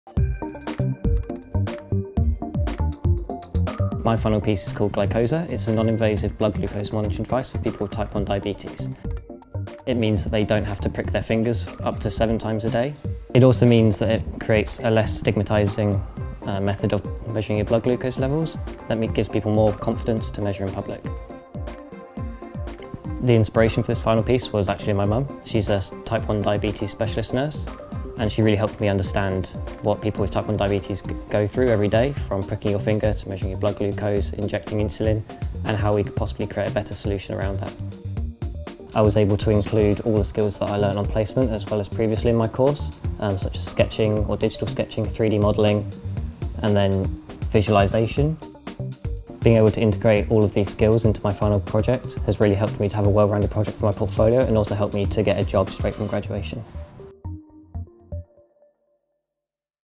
Product Design at Coventry University (Student Project)
Mdes Product Design graduate Tom Harding talks about the
 non-evasive blood glucose monitoring devise he has designed.